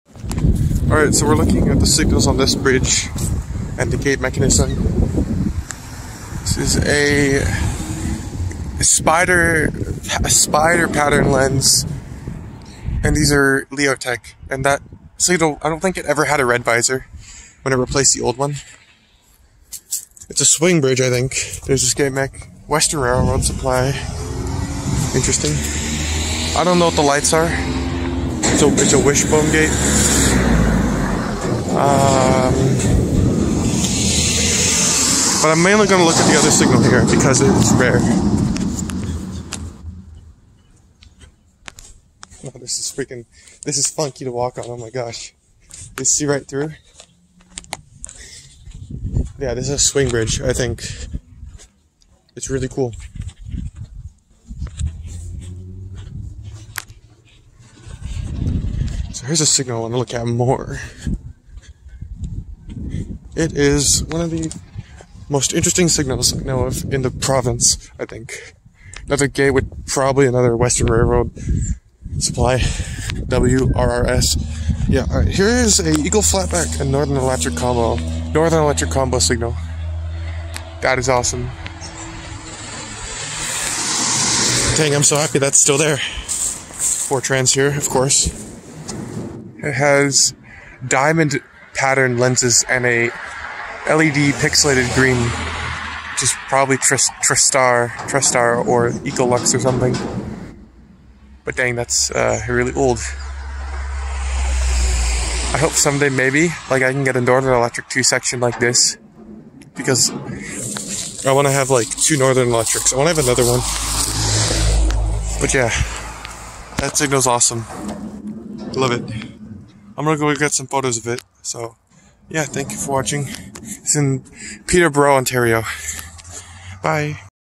*Extremely Rare* Eagle Flatback/Northen Electric combo signal
Originally filmed on April 10, 2022.

Revisited the old Eagle Flatback/Northen Electric combo signal! Glad it's still in service.
This is at the swing bridge at by lock 20 at Peterborough Ontario.

Stay happy & safe! ^w^